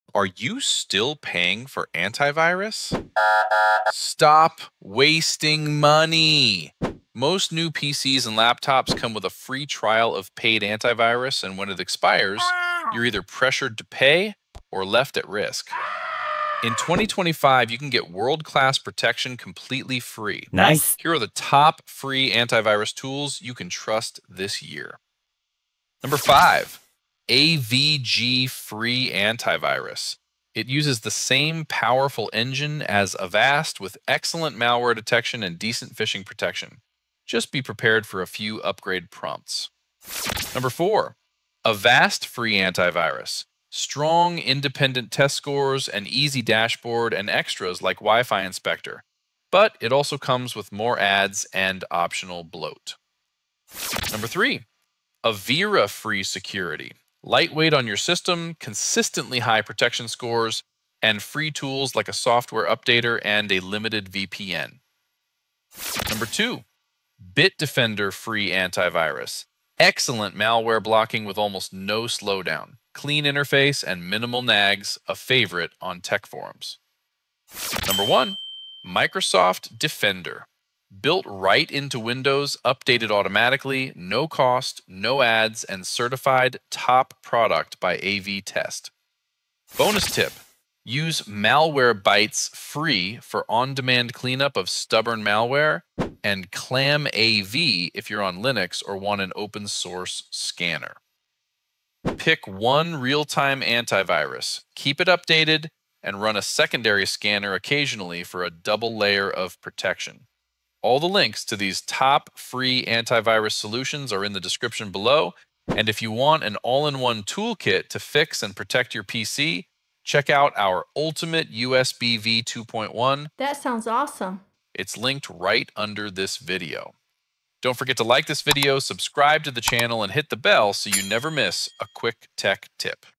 Stop Paying for Antivirus! Top 5 Free Antivirus Programs for 2025
Are you still paying for antivirus? Most new PCs come with a trial of paid antivirus software, and when it expires you’re either pressured to pay or left at risk. In this video, we reveal the Top 5 Free Antivirus Programs for 2025 that deliver world-class protection without the cost.

We’ll break down Microsoft Defender, Bitdefender Free, Avira Free, Avast Free, and AVG Free — plus bonus tools like Malwarebytes Free and ClamAV — so you know exactly which ones to trust.

💾 Get the Ultimate USB v2.1 — the all-in-one PC repair and protection toolkit used by IT pros.